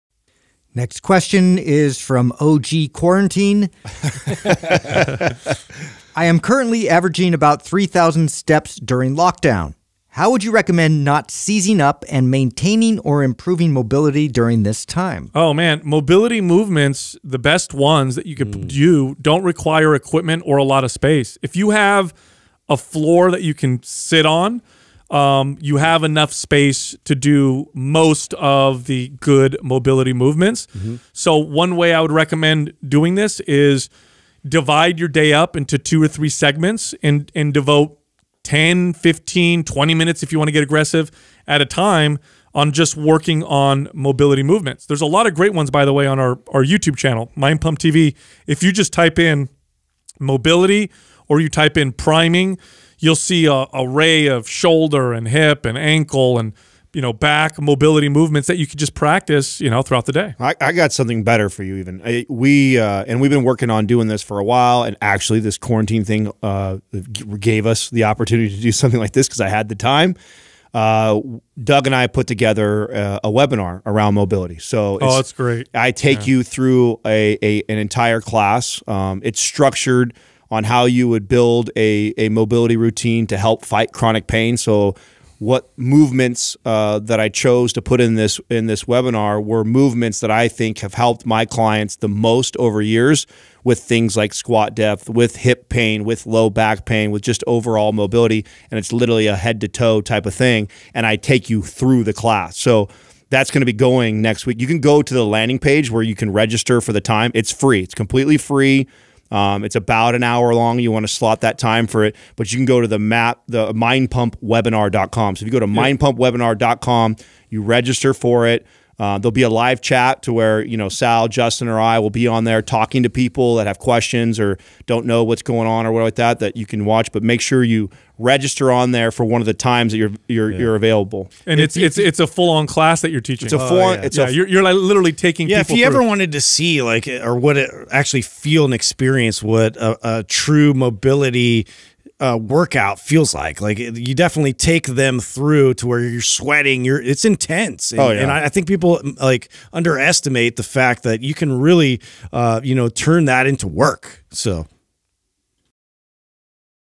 Ways to Maintain & Improve Mobility While Sheltering in Place
In this QUAH Sal, Adam, & Justin answer the question “I am currently averaging about 3,000 steps during lockdown. How would you recommend not seizing up and maintaining or improving mobility during this time?”

* April Promotion: MAPS Prime/Prime Pro ½ off! **Code “PRIME50” at checkout**
* Special Promotion: MAPS Anywhere ½ off!! **Code “WHITE50” at checkout**